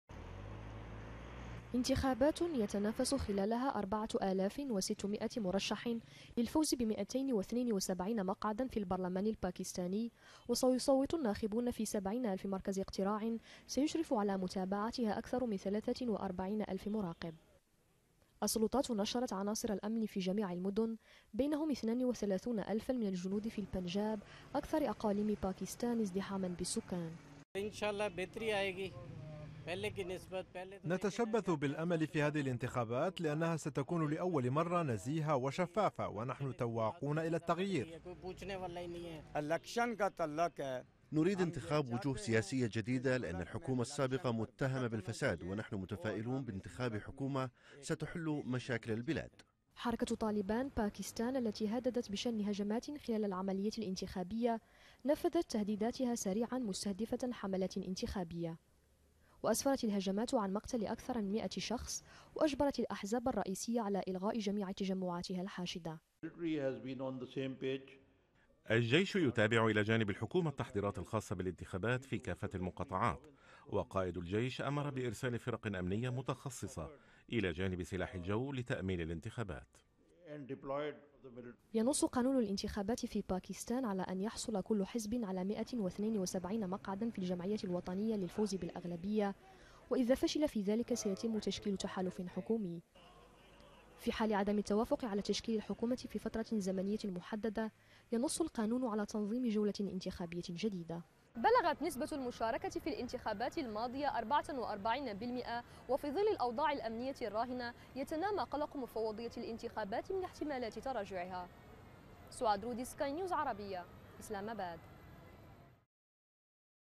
الباكستانيون يستعدون لانتخابات تاريخية
يتوجه الناخبون الباكستانيون يوم السبت، للتصويت في انتخابات توصف بالتاريخية، لاختيار ممثليهم في البرلمان والمجالس المحلية، بعد أن أنهت حكومة مدنية لأول مرة في تاريخ البلاد، ولايتها دون انقلاب عسكري. لمشاهدة المزيد، يرجى زيارة صفحة الفيديو على الموقع الإلكتروني لسكاي نيوز عربية من خلال الضغط على الرابط التالي: